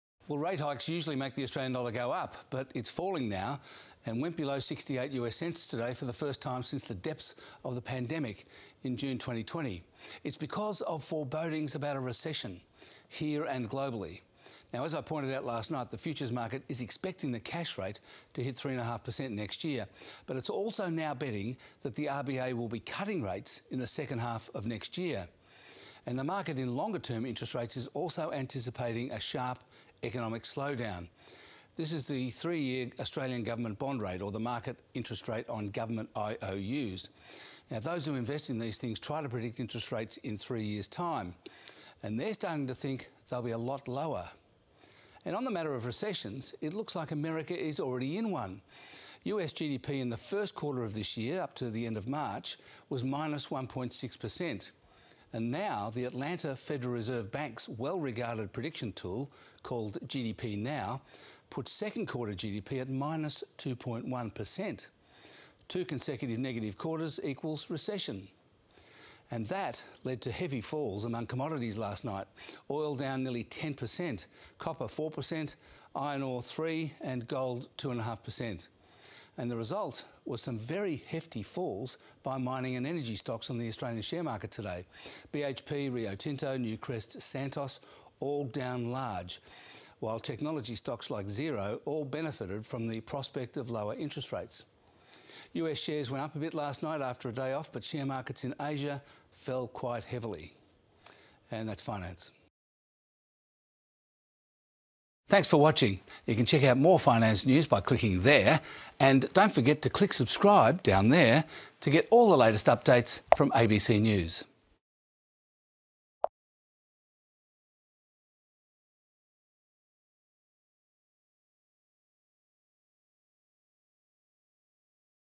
ASX slumps as global recession anxiety plagues global markets and oil prices | Finance Report
Renewed worries about a global recession have led to sharp falls in the Australian dollar, commodity prices, and global markets.